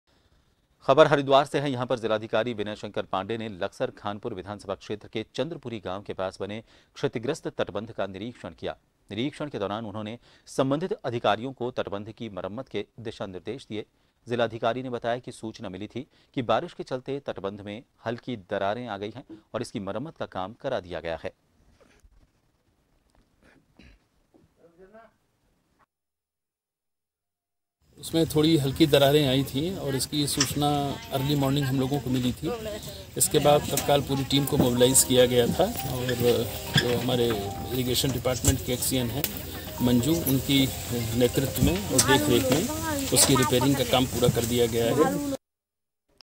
Haridwar | जिला कलेक्टर ने किया क्षतिग्रस्त का निरिश्रण,अधिकारियों को दिए मरम्मत संबधी दिशा-निर्देश
News Credit-MKP